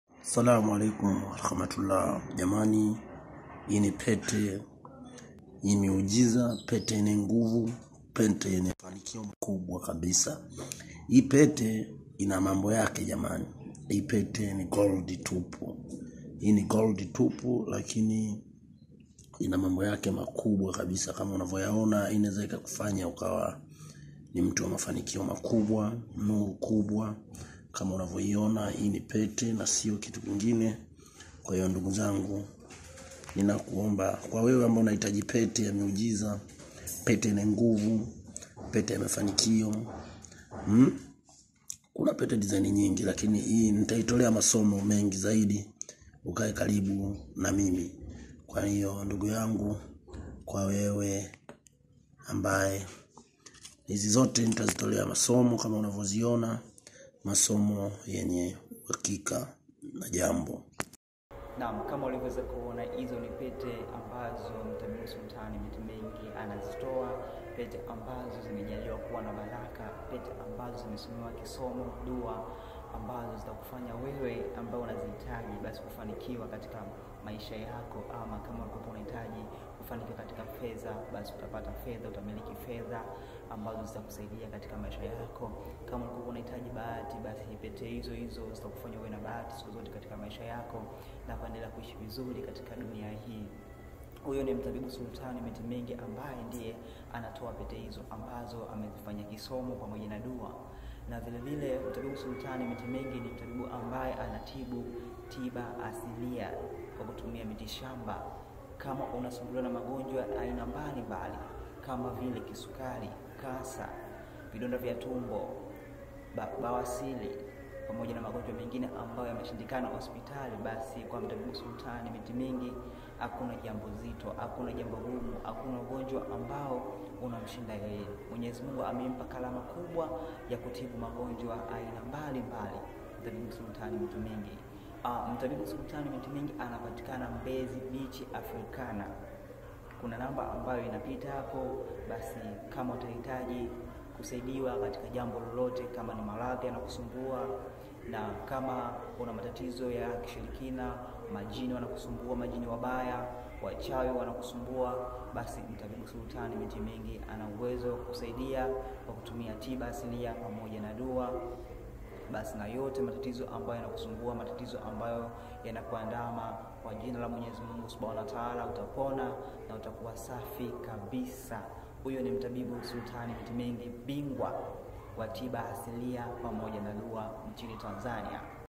SOMO LA PETE ZA BAHATI ,PETE ZA UTAJIRI NA JINSI YA KUZITUMIA ILI ZIKULETEE MAFANIKIO
mtabibu sultan mitimingi